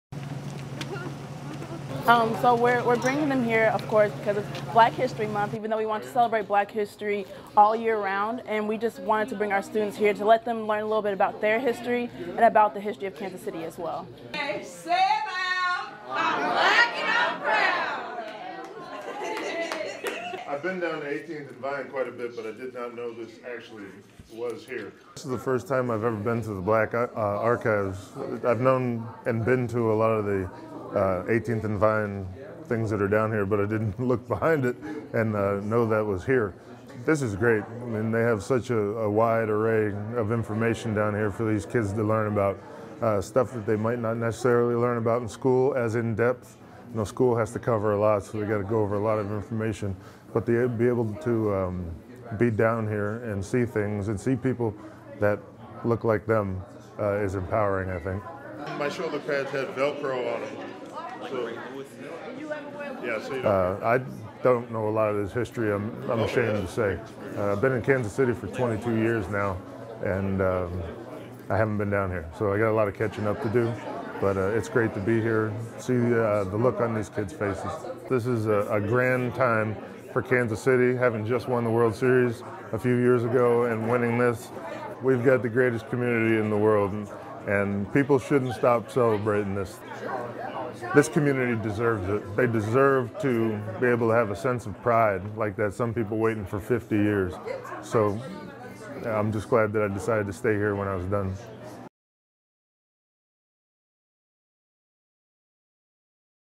Former Chief Eric Hicks tours Black Archives museum with KC middle schoolers
Former Kansas City Chiefs defensive end Eric Hicks tours the Black Archives of Mid-America with students from Central Middle School on Wednesday morning, Feb. 19, 2020 in Kansas City, Missouri. More on the Kansas City Chiefs

Video by Shelly Yang | The Kansas City Star